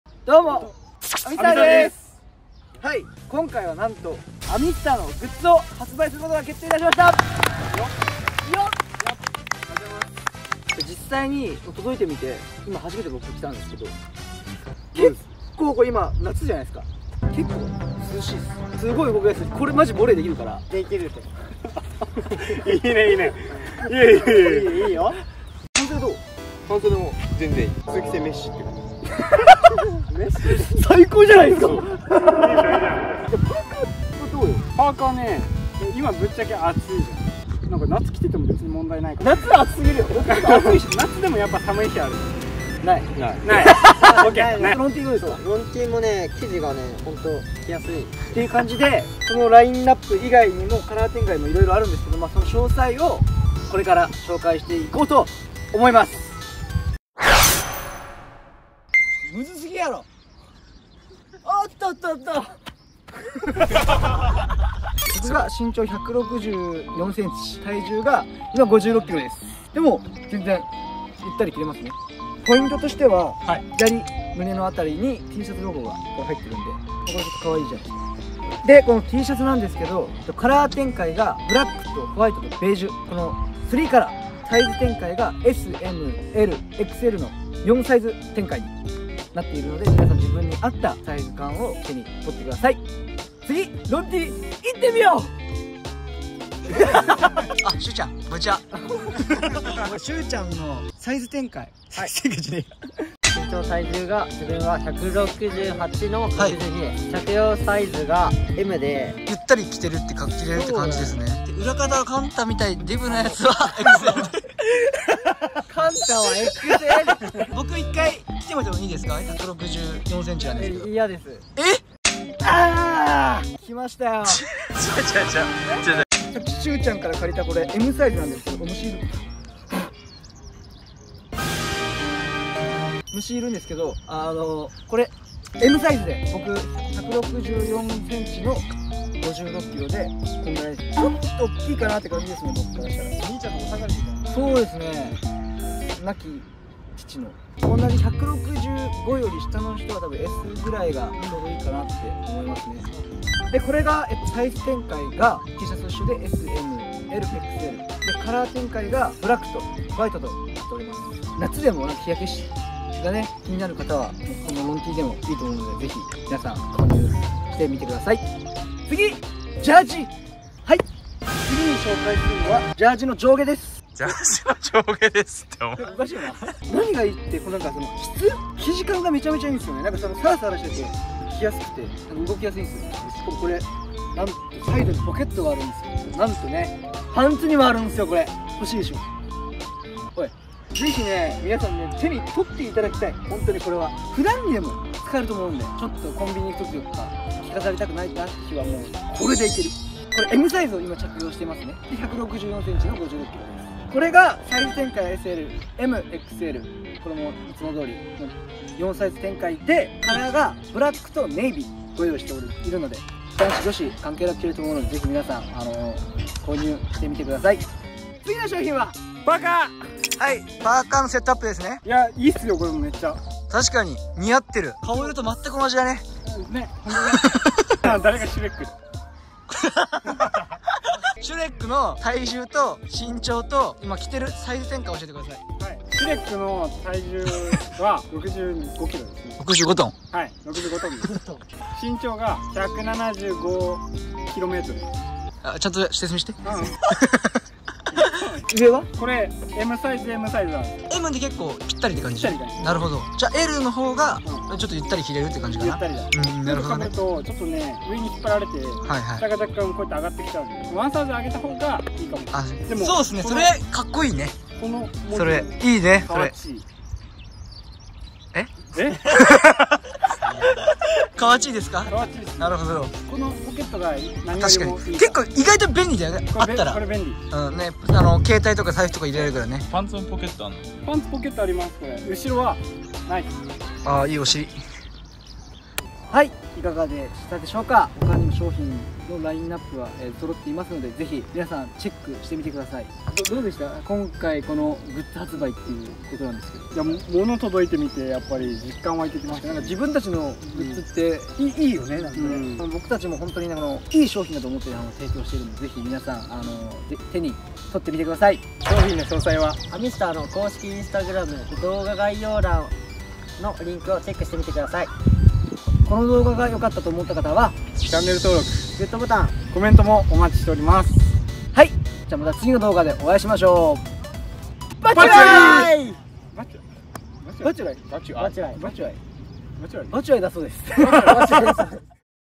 【重大報告】アミスター初のグッズが発売されることになりました！！！
こんにちは！アミスターです⚽️
今回アミスター初のグッズを発売することになりました🔥
メンバー一同デザインから時間をかけて、皆様にやっと報告できることが嬉しいです
どの商品もオススメできる商品となっておりますので、ぜひお手に取ってみてください!!

夏にぴったりのTシャツから、秋冬用のセットアップまでご用意しておりますので
ぜひ最後まで動画をご覧ください👀

しゅうちゃんのSNS⏬